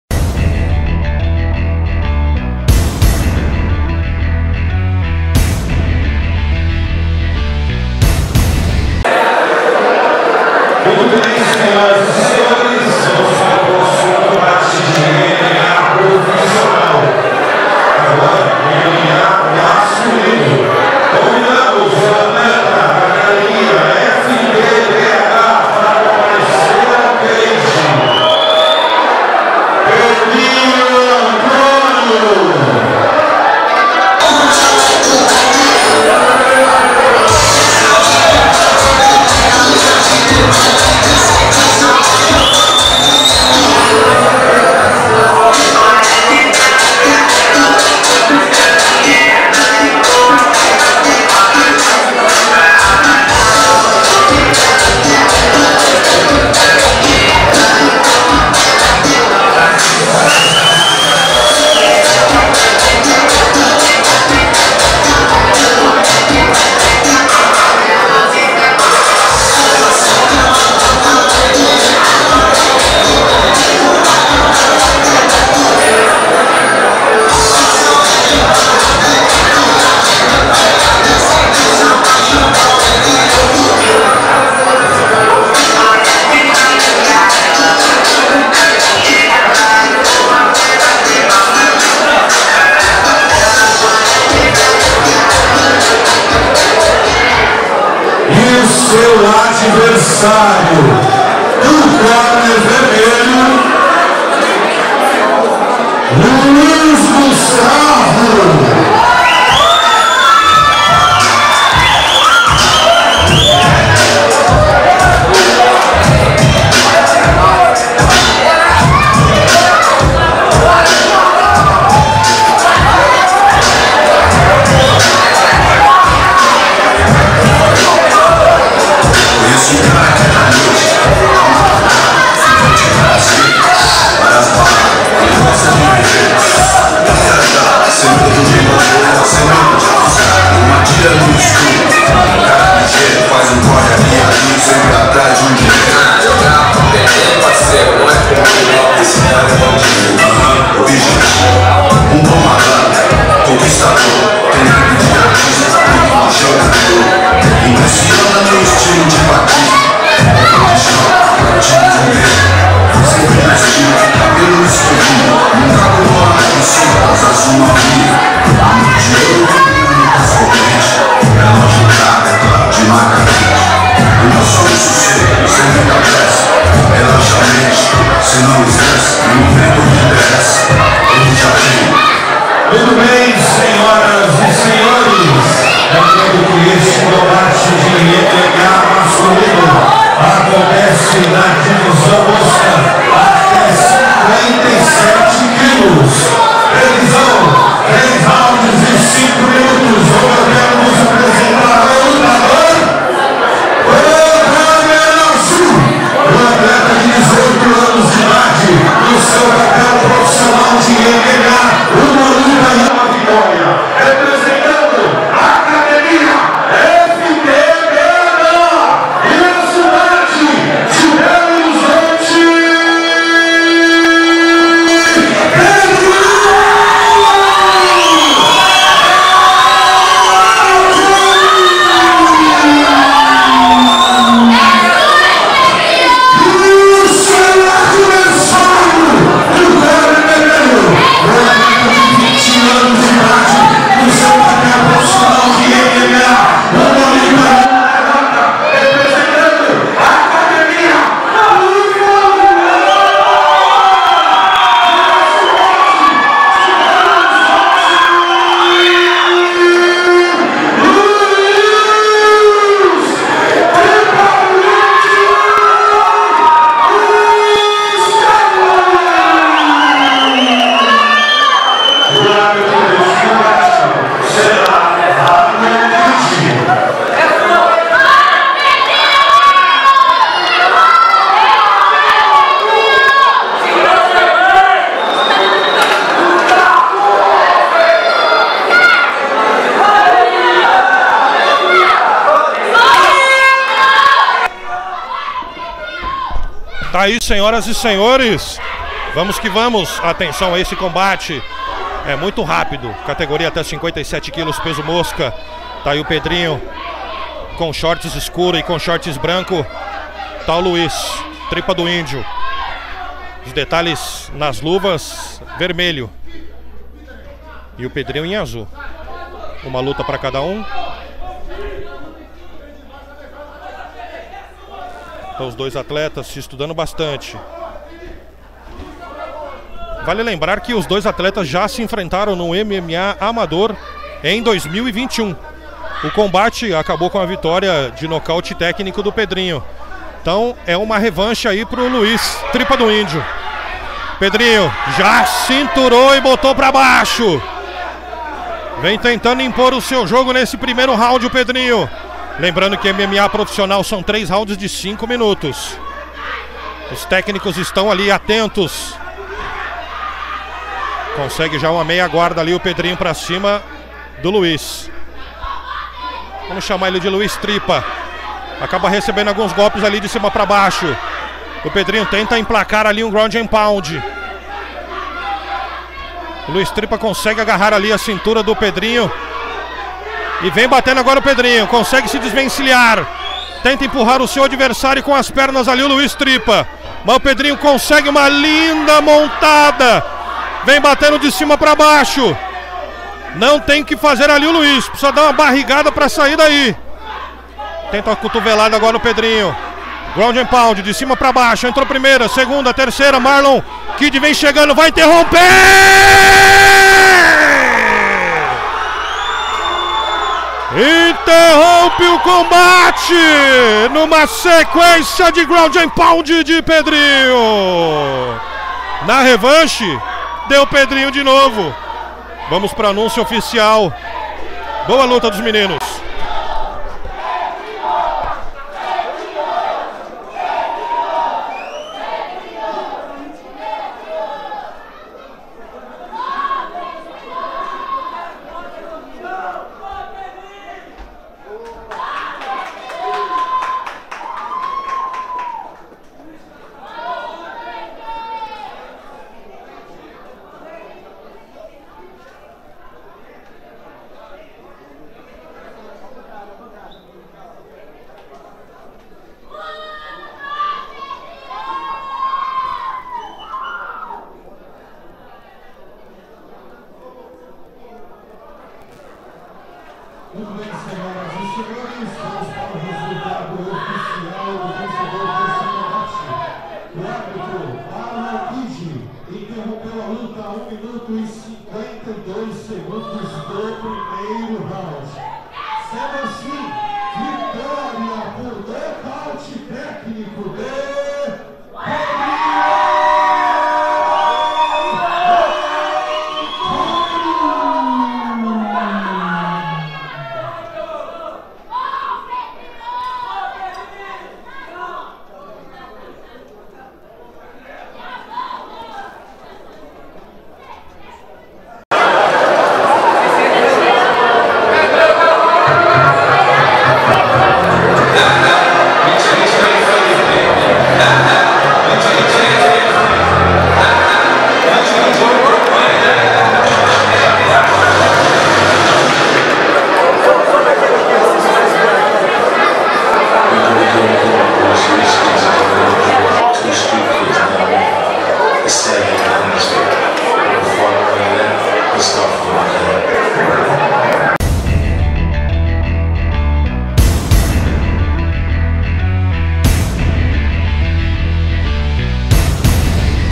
Pedrinho vs Luis Tripa - FT Fight Championship MMA 15
Em combate de MMA profissional até 57kg, Pedrinho Antonio (FTBH) venceu Luis Tripe (Búfalo BH) por nocaute técnico aos 1:53 do primeiro round.